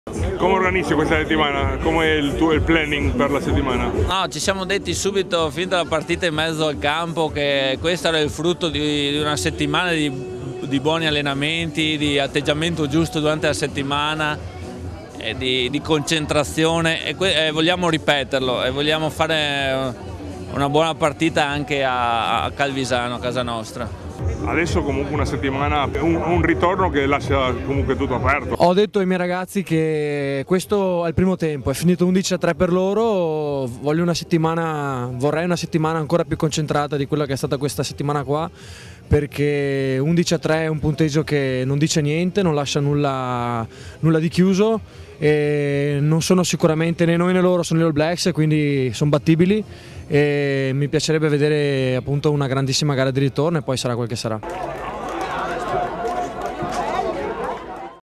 160518 Brunello e Conforti post match Padova vs Calvisano play off andata
Aspettando il ritorno play off di domenica Calvisano vs Padova , il pensiero dei Massimo Brunello head coach del Calvisano e Federico Conforti capitano del Petrarca.